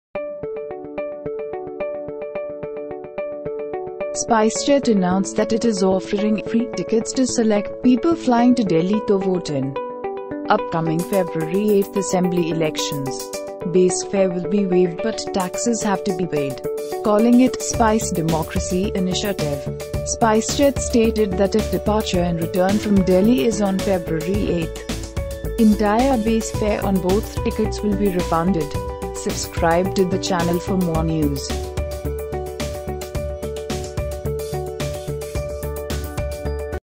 SpiceJet offers free tickets to those travelling to Delhi to vote on Feb 8
SpiceJet announced that it is offering "free" tickets to select people flying to Delhi to vote in upcoming February 8 Assembly elections. Base fare will be waived but taxes have to be paid. Calling it "SpiceDemocracy" initiative, SpiceJet stated that if departure and return from Delhi is on February 8, entire base fare on both tickets will be refunded.